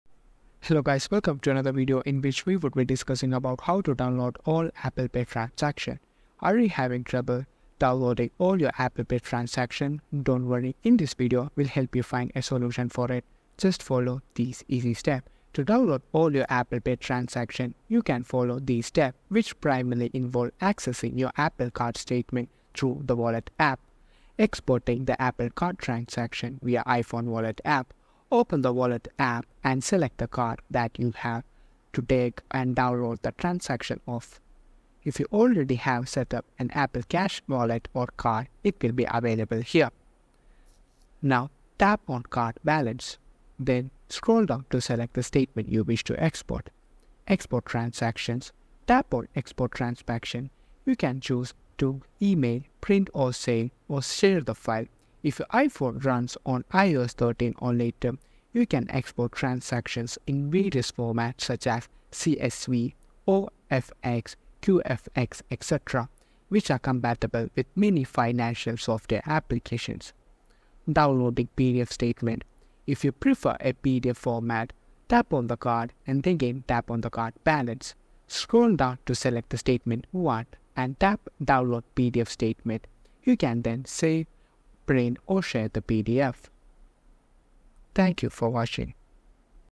How To Download All Apple Pay Transactions
Learn how to download all your Apple Pay transactions for record-keeping and analysis. This video will guide you through the steps to access and export your transaction history.